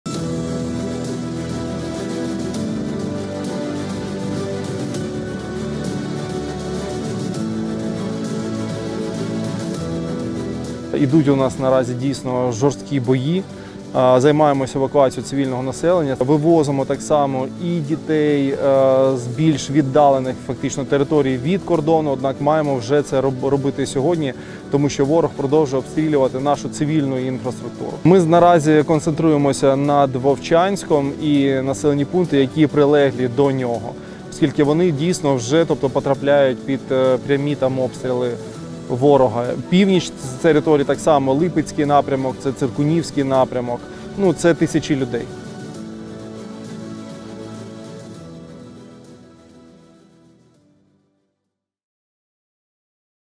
На Харківщині триває евакуація з територій, які обстрілює ворог
Начальник Харківської ОВА Олег Синєгубов відвідав прикордонні населені пункти, де наразі відбувається евакуація населення.